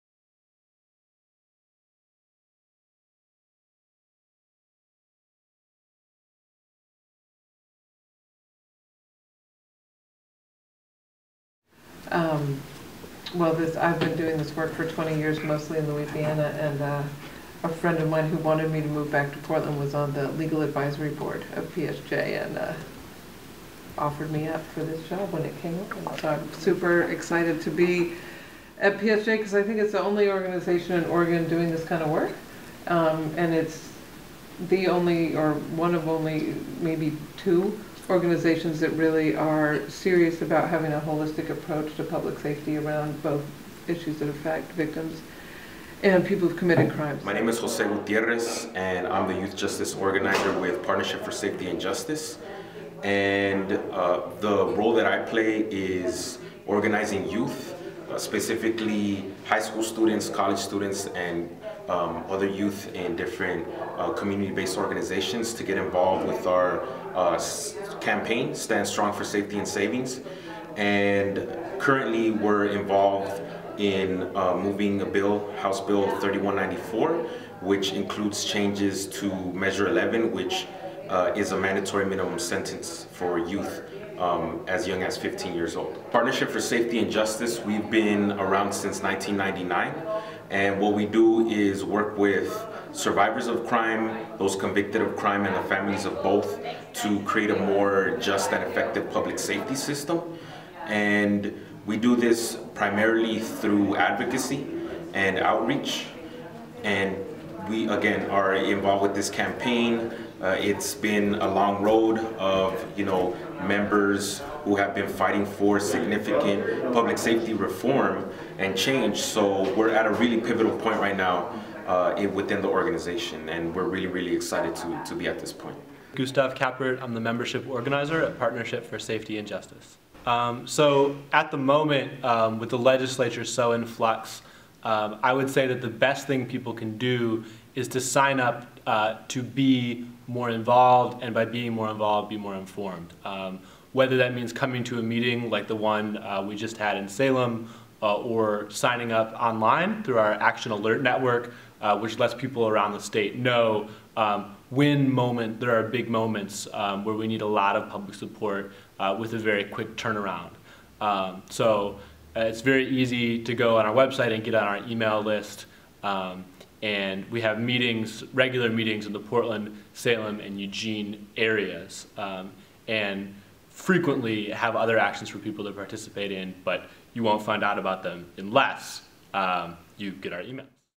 PSJ Action Team Meeting - (YWCA Salem, Oregon)
PSJ (Partnership for Safety and Justice) Salem Action Team Meeting last night members came to get informed on the facts in the struggle for public safety reform and be prepared to raise our voices on the issue. To get involved sign up for our Action Alert on the website link provided below.